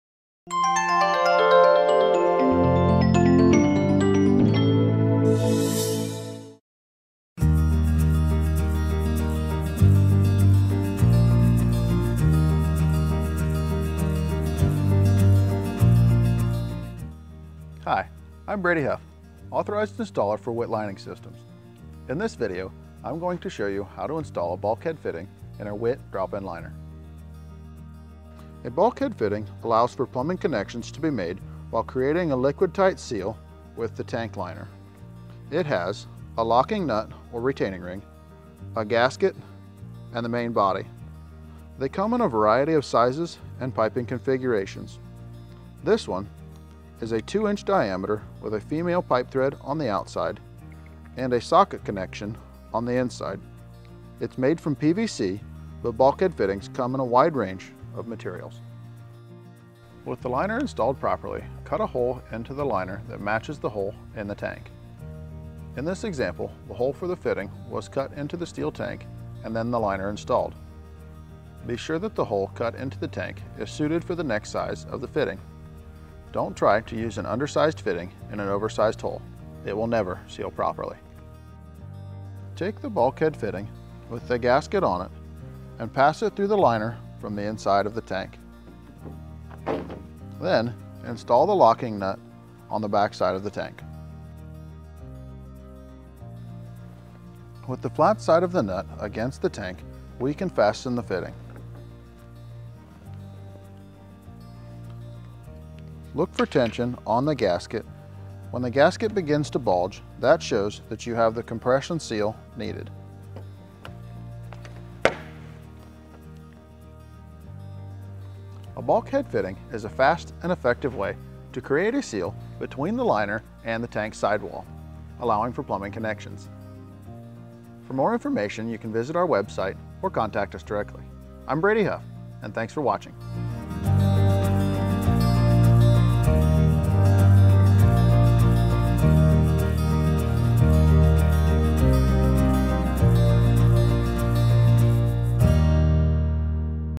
Bulkhead Fitting Installation with a Witt Tank Liner
A bulk-head fitting allows for plumbing connections to be made while creating a liquid seal between the tank wall and the flexible drop in liner.
A bulk-head fitting consists of retaining ring, a gasket, and the main body.  The fittings come in a variety of sizes and piping configurations.  Bulk head fittings are made from a wide range of materials and be sure to choose one that is compatible with the solution application.
With the liner properly installed, use a utility knife to cut a hole in the liner to match the hole in the tank.  Be sure the hole cut into the tank is suited for the size of the fitting.  A tank hole to small will not allow the fitting to pass through and a hole cut too big will not allow a liquid tight seal to be formed.  With the gasket of the fitting on the inside of the main body pass the fitting through the hole in the liner and the tank.  Apply the locking nut and tighten.  When the gasket of the fitting starts to bulge it indicates that enough pressure has been applied and a sealed connection has been made.